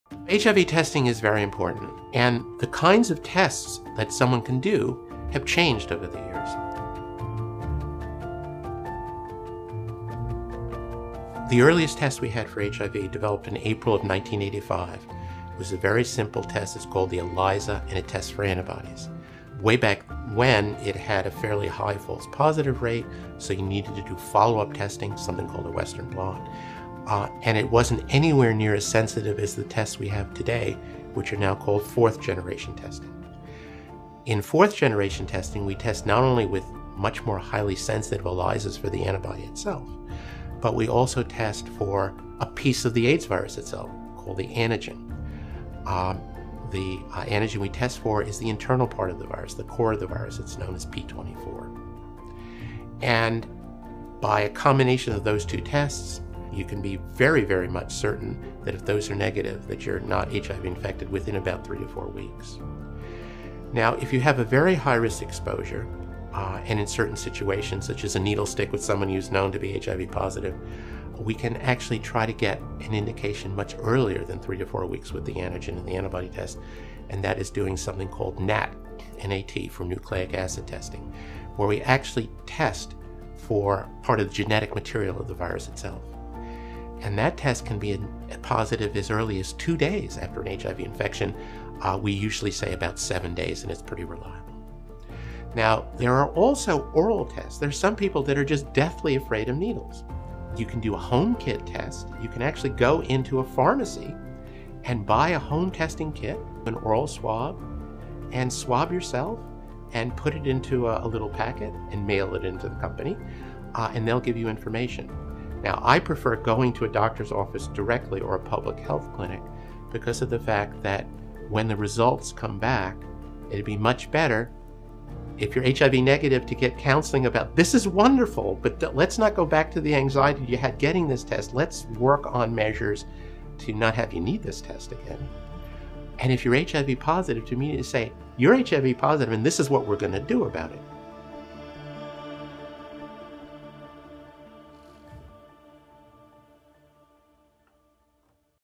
Different Types of HIV Tests and How They Work | GoodRx
In this video, Jeffery Laurence, MD, talks about the different types of HIV tests and how they work to detect HIV in days or weeks.